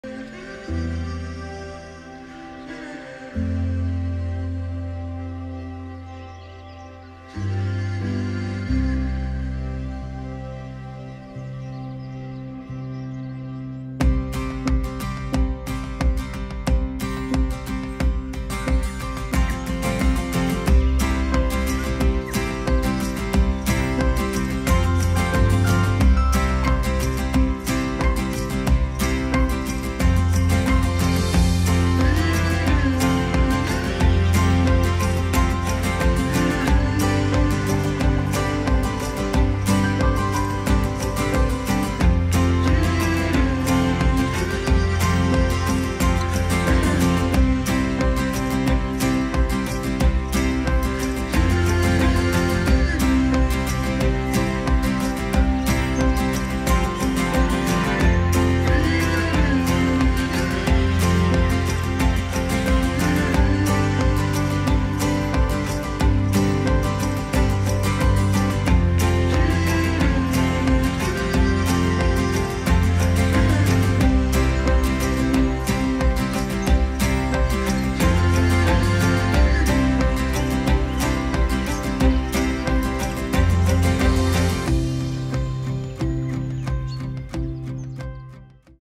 Retro Beach Vibes
Located just a short stroll from the beautiful local beach and boasting impressive ocean views this 4 bedroom home is the perfect mix of beach bach, modern features and super cool styling.